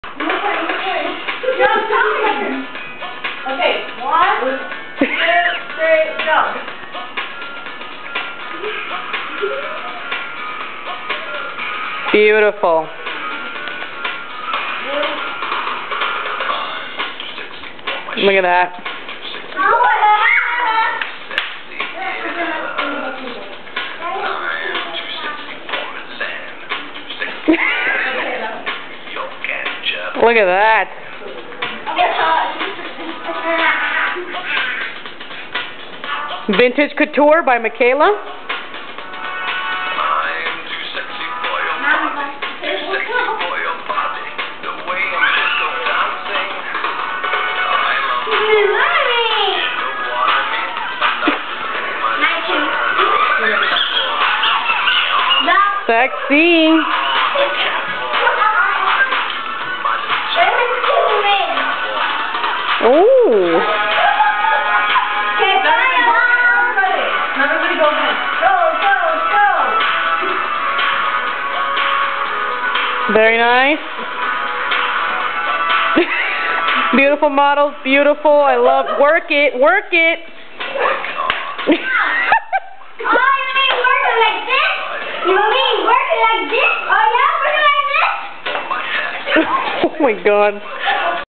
PJ Runway Show
nothing is more fun that having your own pajama party with its own fashion show!